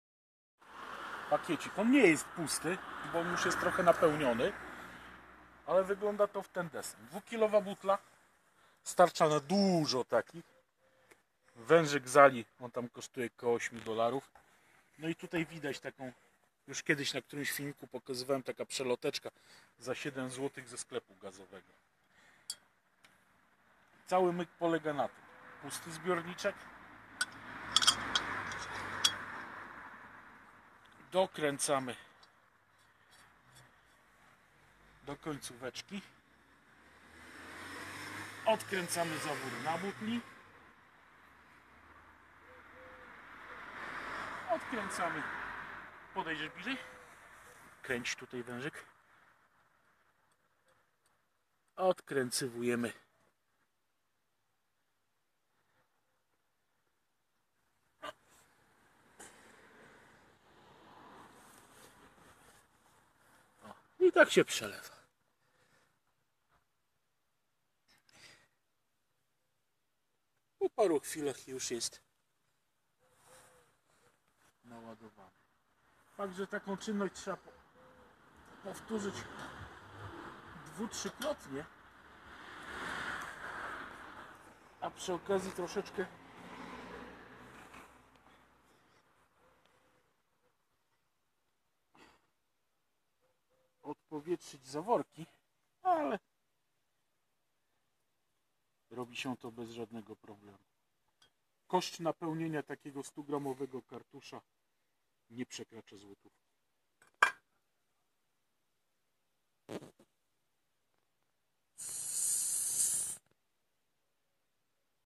napełnianie kartusza z gwintem 7/16'
tak napełniam propan butanem kartusze do turystycznych kuchenek. Skończyły się czasy szczypania się na kartusz ( koszt napełnienia takiego 100g kartusza to około 90groszy)

wymiana butli 11kg z dowozem 49pln
2kg turystyczna 9pln

teoretycznie z 11kg butli nabijasz 22 500g kartusze.
 koszt nabicia 2,25 koszt kartusza ( najtańszy z gwintem 7/16' (10mm) 500g to 15pln)

w 400ml wchodzi ok 250g gazu, czyli nabicie kosztuje niewiele ponad 1,20pln

Konieczne inwestycje

Butla gazowa. Cena od 50pln za 0,5kg turystyczną do ok 120pln za 11kg.
Jeżeli kożystamy z butli turystycznych z gwintem złącza 3/8'L ( lewy zewnętrzny) potrzebna będzie taka przelotka
 Jeżeli butli stacjonarnej z gwintem złącza 1,2'L (lewy zewnętrzny) nie będzie potrzeba żadnych adapterów.

Taki wężyk pasuje do kartuszy z gwintem 7/16L (10mm) czyli najpopularniejszych nakręcanych. Jeżeli chcemy napełniać cylindryczne kartusze
400gramowe potrzeba jeszcze takiego adaptera

Wszystkie linki są linkami przykładowymi. Przy odrobinie chęci można te części kupić taniej ( wężyk i adapter na kartusze 400g na Aliexpresie.
 Koszt wężyka około 6-7$ adaptera 3-4$).Przy zakupie z ali trzeba tylko pamiętać żeby zaznaczyć że
 chcemy taki do europejskich butli (azjaci używają bytki z gwintem wewnętrznym, u nas z gwintem zewnętrznym)

Przelotka z butli turyztycznej na wężyk do przelewania jest dostępna w praktycznie każdym sklepie hydrauliczno gazowniczym za 7-9pln.

Jako że ten filmik jest jedynie poglądowy przed próbami napełniania proszę zapoznać się z dokładną instrukcją którą przedstawił Szeryf54 na swoim kanale w tym filmiku . Szczególnie ważne jest nieprzekraczanie ilości przelewanego gazu.

Polecam również przejrzenie całego jego kanału bo jest tam sporo materiału na temat kuchenek turystycznych i napełniania kartuszy